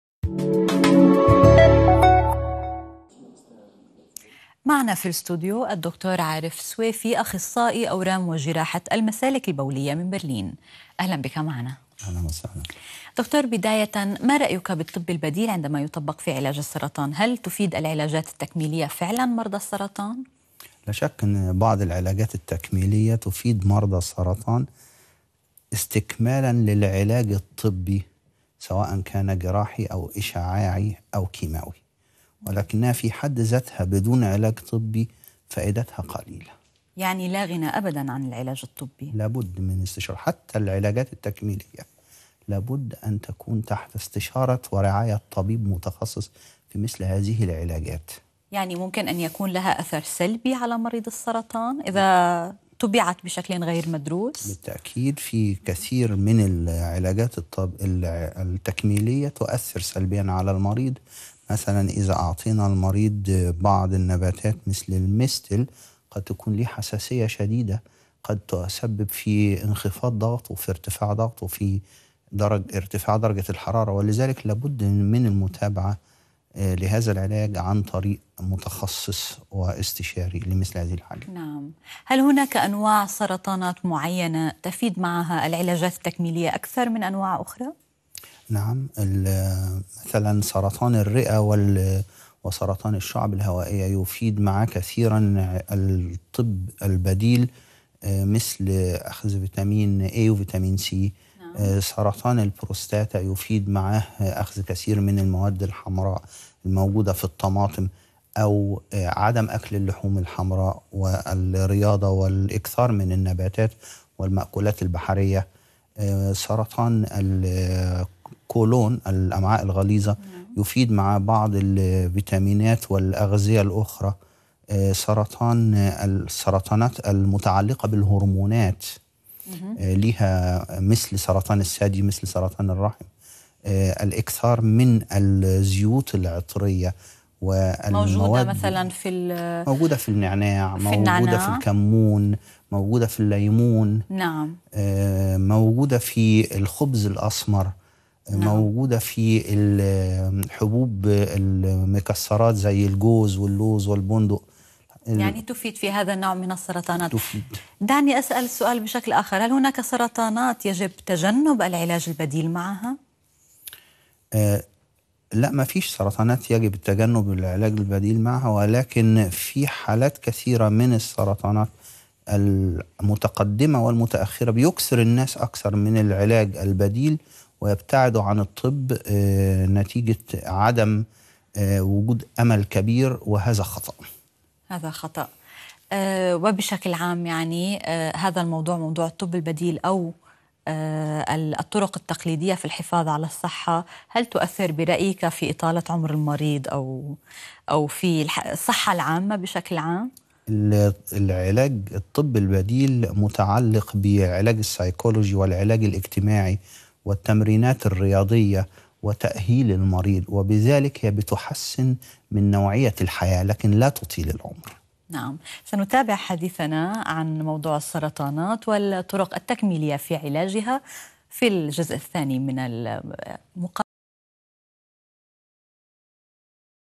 الطب البديل وعلاج السرطان؟ | صحتك بين يديك
ضيف حلقتنا هو الدكتور عارف سويفي